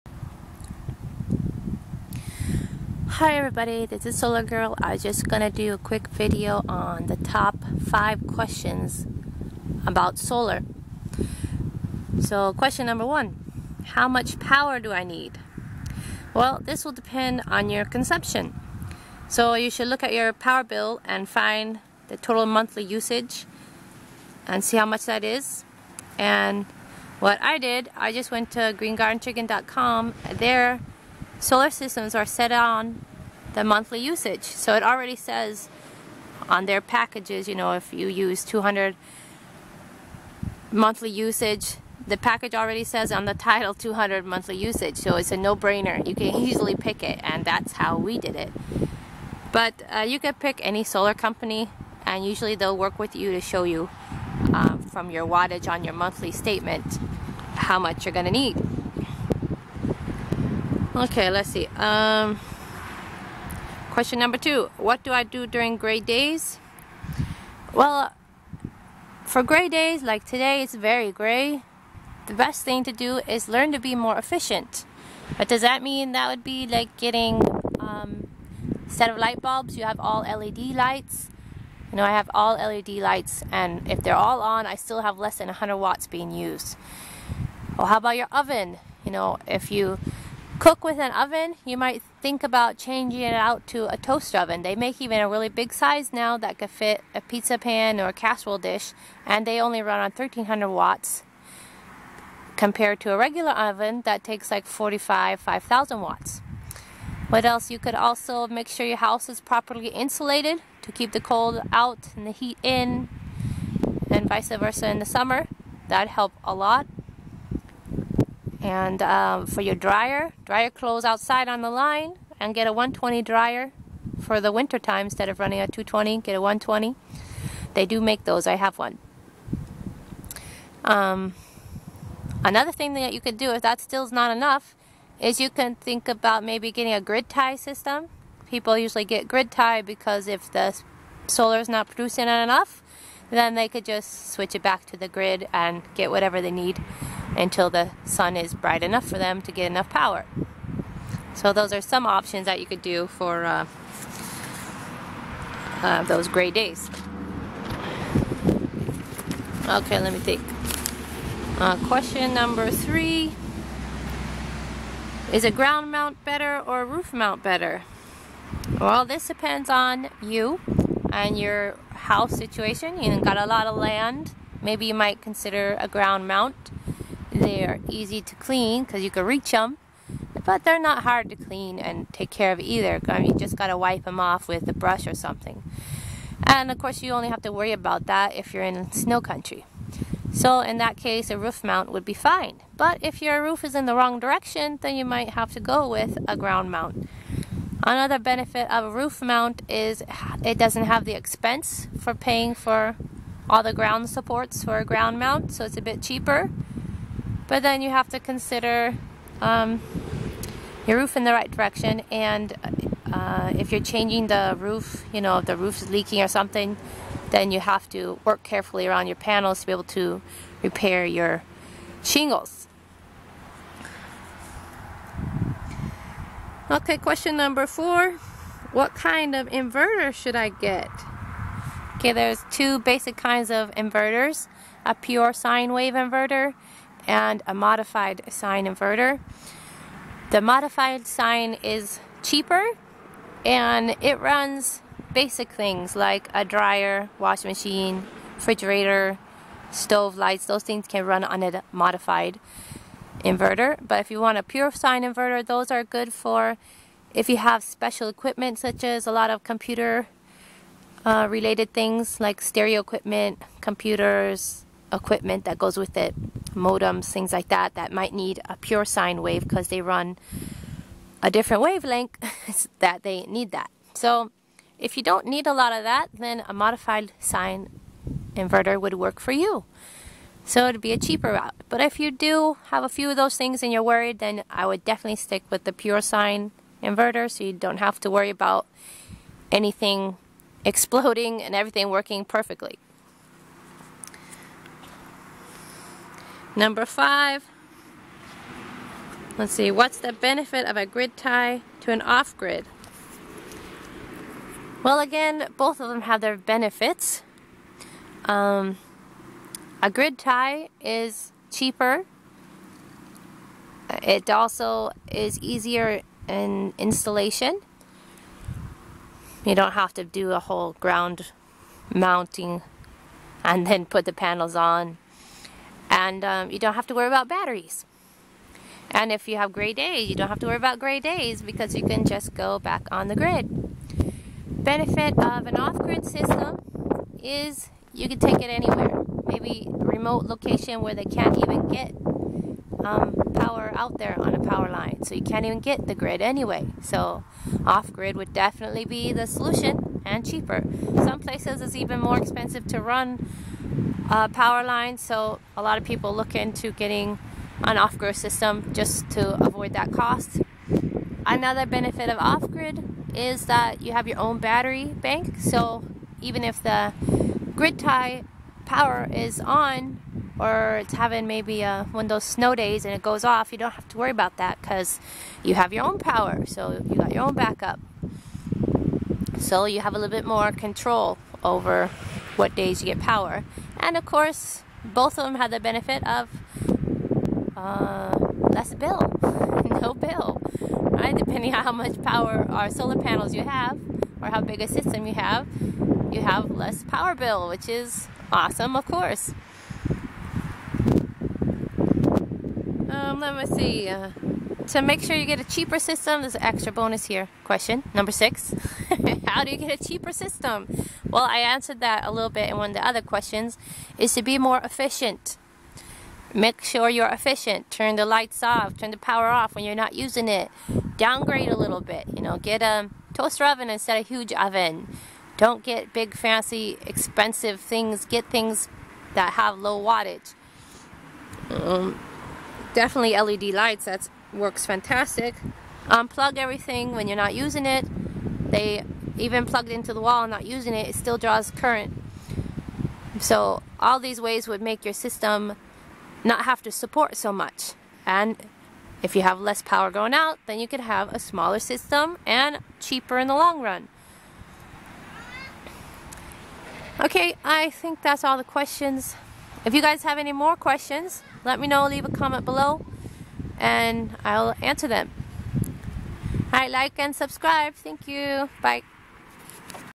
Should I Buy Solar? | Top 5 Questions
Why should I buy Solar?
How much power do I need?
What  do I do during grey days for power?
Ground mount or roof mount better for solar?
What kind of inverter should I get?
Whats the benefit of a grid tie or off grid system?

solar questions
off grid solar